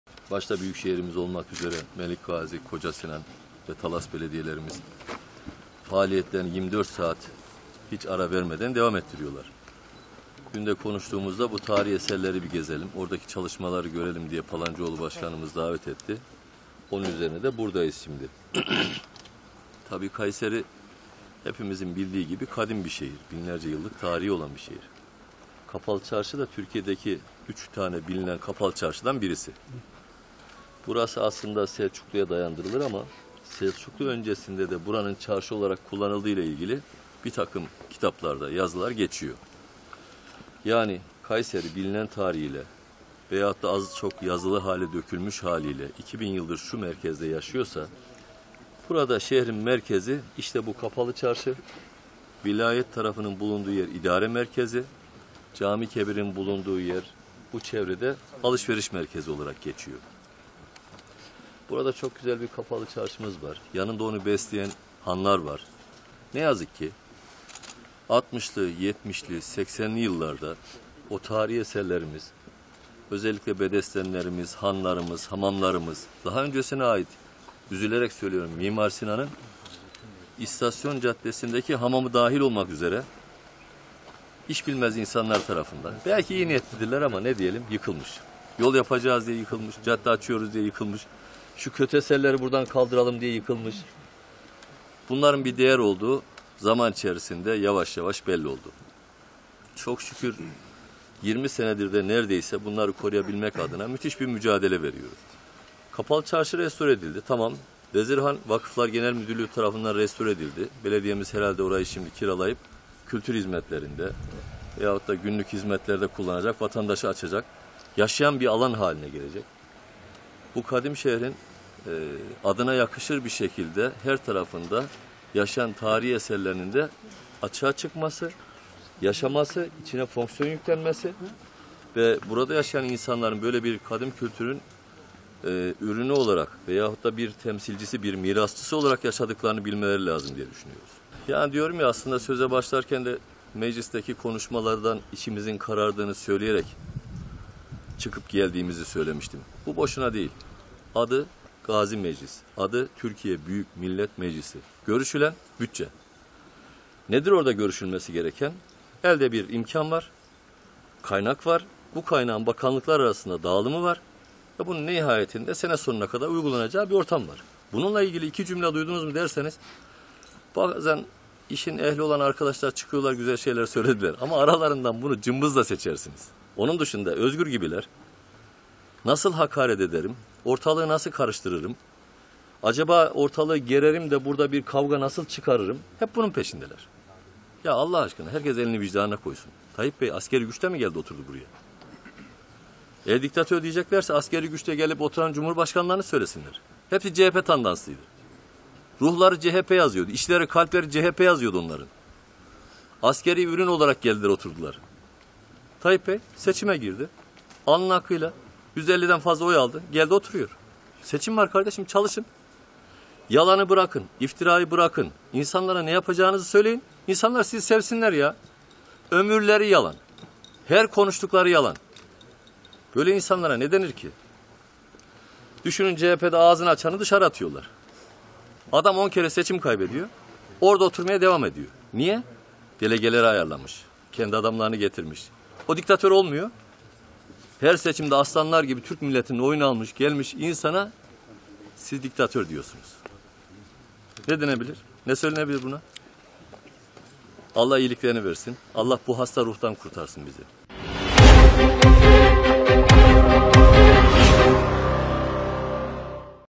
AK Parti'li Özhaseki'den CHP'li Özel'e 'diktatör' tepkisi
AK Parti Genel Başkan Yardımcısı Mehmet Özhaseki, CHP Grup Başkanvekili Özgür Özel'in Cumhurbaşkanı Recep Tayyip Erdoğan’a yönelik 'diktatör' sözlerine ilişkin, "Tayyip Bey buraya askeri güçle mi geldi? 'Diktatör' diyecekler ise askeri güçle gelip oraya oturan Cumhurbaşkanlarına söylesinler" dedi.

AK Parti Genel Başkan Yardımcısı Mehmet Özhaseki, Kayseri Kapalı Çarşı'da restorasyon çalışmaları devam eden tarihi Vezir Han ile Gön Han'da incelemelerde bulundu. Basın mensuplarının sorularını cevaplandıran Özhaseki, CHP Grup Başkanvekili Özgür Özel’in Cumhurbaşkanı Recep Tayyip Erdoğan'a yönelik 'diktatör' sözlerine ilişkin "Özgür gibiler 'nasıl hakaret ederim, ortalığı nasıl karıştırırım, ortamı nasıl gererim de burada nasıl kavga çıkartırım' bunun peşindeler. Herkes elini vicdanına koysun; Tayyip Bey buraya askeri güçle mi geldi? 'Diktatör' diyecekler ise, askeri güçle gelip oraya oturan Cumhurbaşkanlarına söylesinler. Hepsi CHP'nin hatasıydı. Tayyip Bey seçime girdi, alnının akıyla yüzde 50'den fazla oy aldı, geldi oturuyor. Yalanı, iftirayı bırakın; insanlara ne yapacağınızı söyleyin, insanlar sizi sevsinler. Ömürleri yalan, her konuştukları yalan" diye konuştu.

CHP'ye yönelik eleştirilerini sürdüren Özhaseki, "Düşünün, CHP'de ağzını açanı dışarıya atıyorlar. Adam 10 kere seçim kaybediyor, hala o koltukta oturmaya devam ediyor. Neden? Delegeleri ayarlamış, kendi adamlarını getirmiş. O diktatör olmuyor, her seçimde aslanlar gibi Türk milletinin oyunu almış gelmiş insana 'diktatör' diyorsunuz. Buna ne denir? Ne söylenebilir? Allah iyiliklerini versin. Allah bizi bu hastalıktan kurtarsın" ifadelerini kullandı.